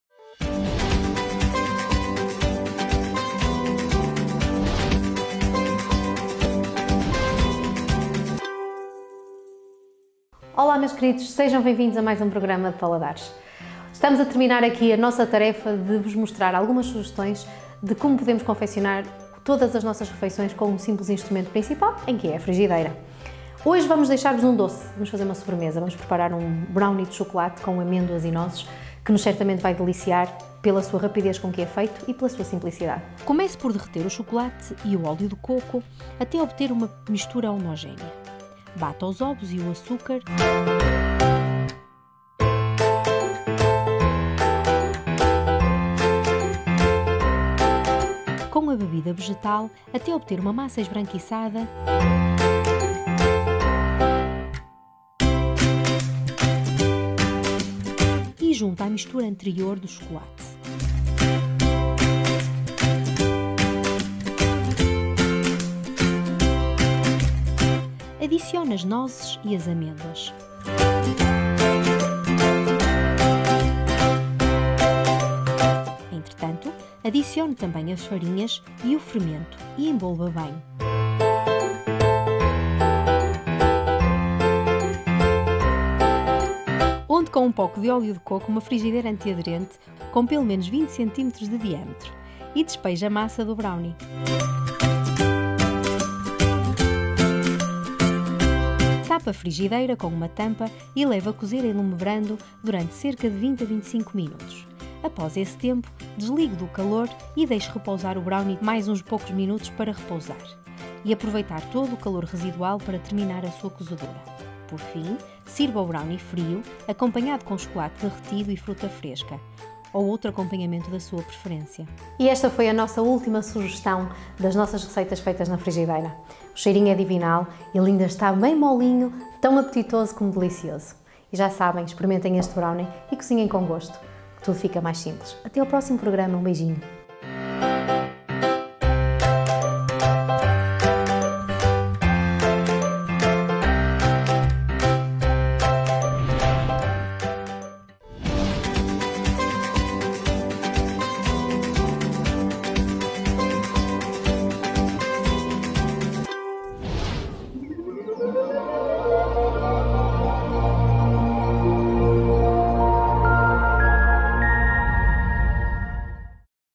Paladares “Brownie na Frigideira” – S02E16
Finalizamos o nosso desafio de algumas receitas todas elas confeccionadas com o principal instrumento e utensílio de cozinha, a frigideira.

Então hoje já iremos deixar aqui aberto o paladar e o vosso apetite para preparar um delicioso  "Brownie  na frigideira".

Cozinhe com gosto e tudo fica mais simples…

Não se esqueçam de SUBSCREVER o nosso canal do youtube, para estarem sempre a par de novos programas e vídeos.